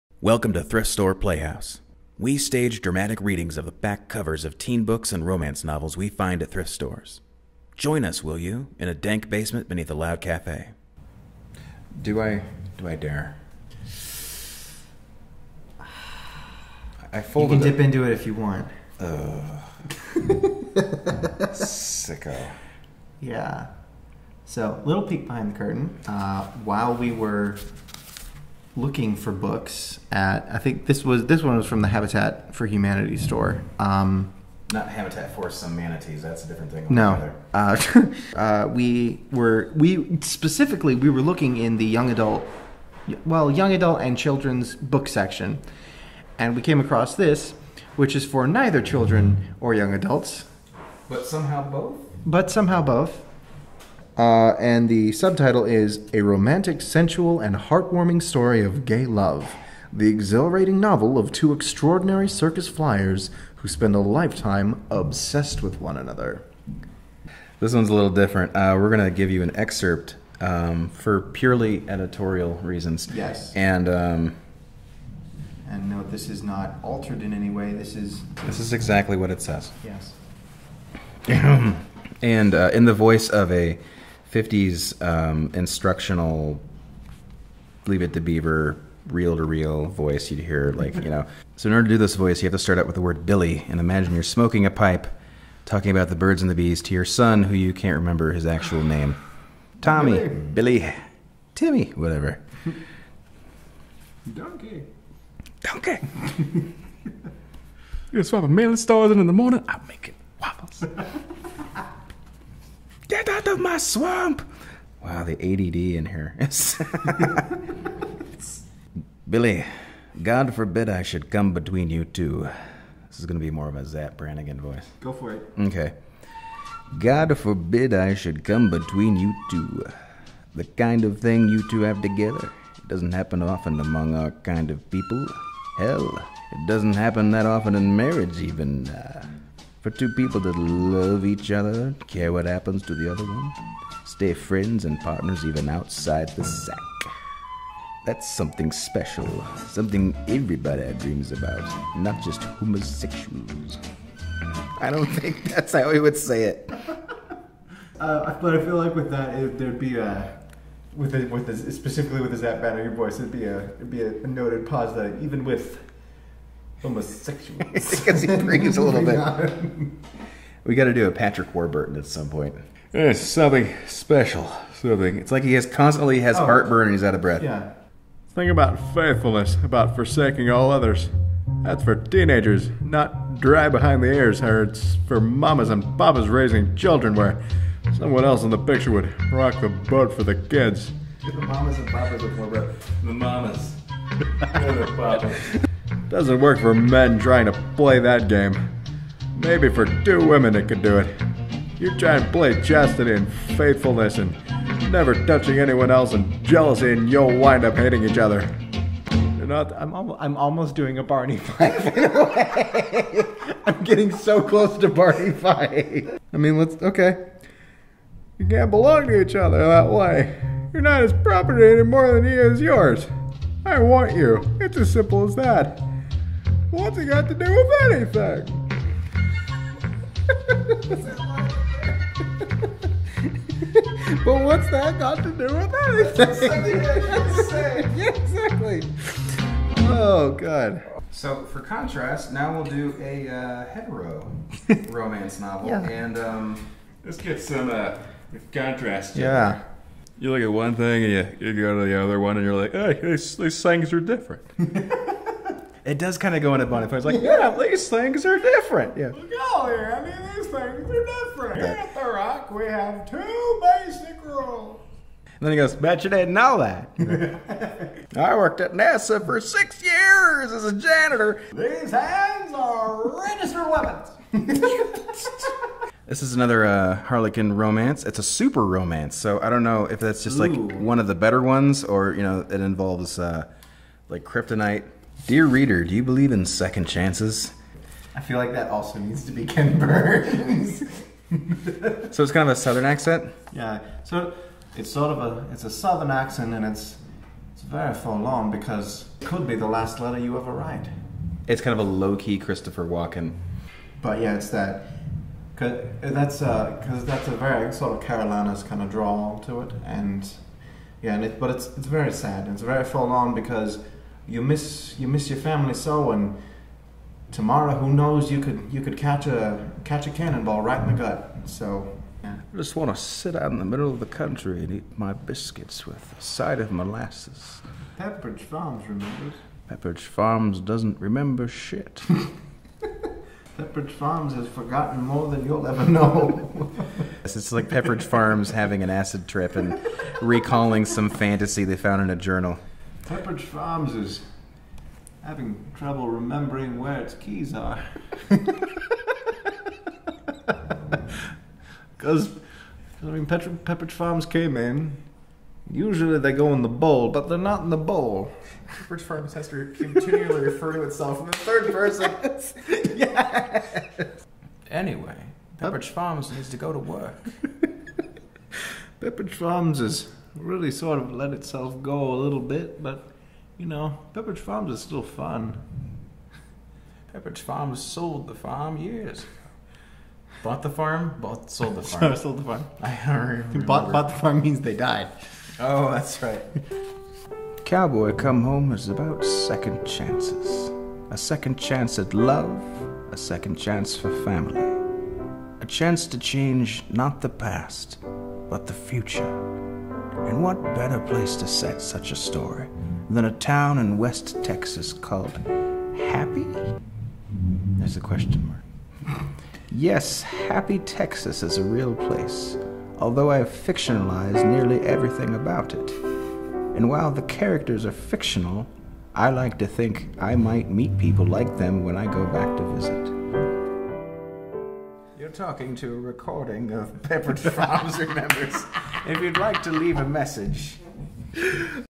Thrift Store Playhouse ep.2 Pepperidge Farms, Barney Fife, and Trapeze Artists
More dramatic readings of the back covers of crappy books from thrift stores.